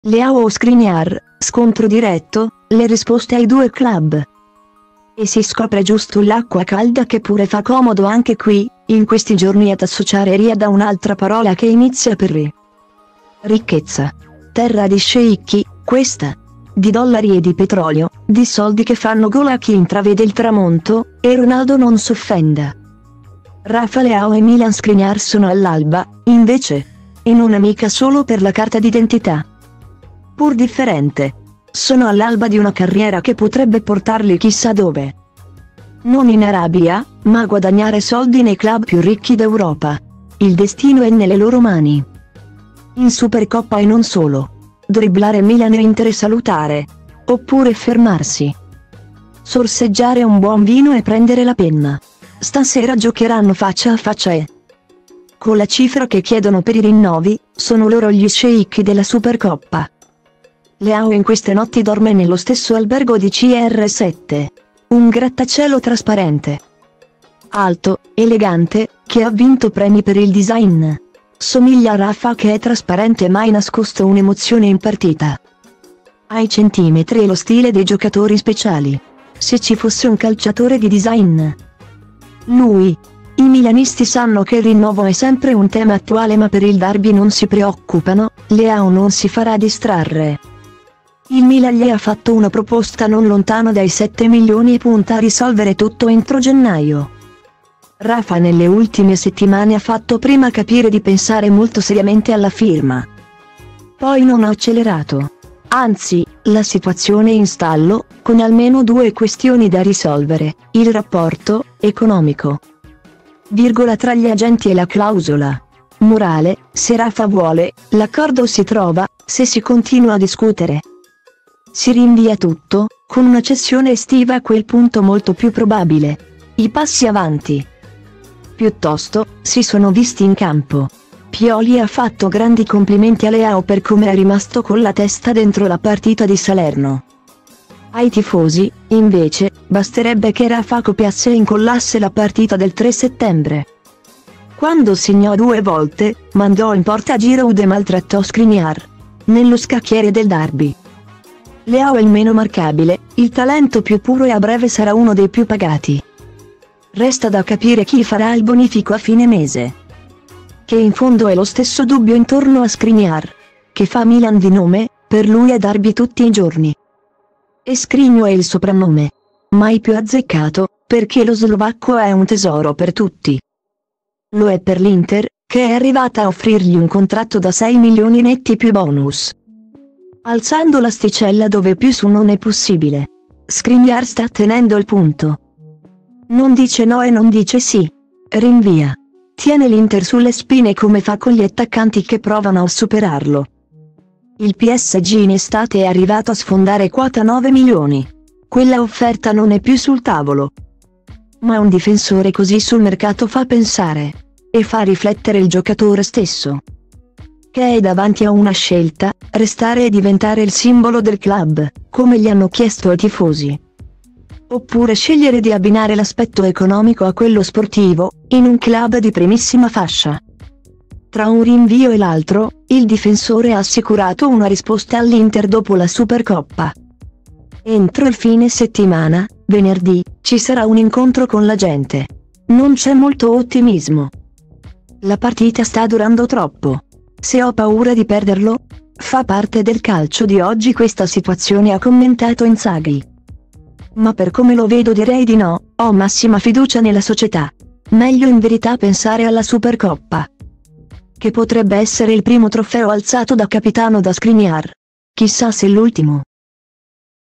Leao o Skriniar: scontro diretto, le risposte ai due club
E si scopre giusto l’acqua calda – che pure fa comodo anche qui, in questi giorni – ad associare Riad a un’altra parola che inizia per “ri”,ricchezza. Terra di sceicchi, questa. Di dollari e di petrolio, di soldi che fanno gola a chi intravede il tramonto (e Ronaldo non s’offenda).Rafa Leao e Milan Skriniar sono all’alba, invece. E non è mica solo per la carta d’identità,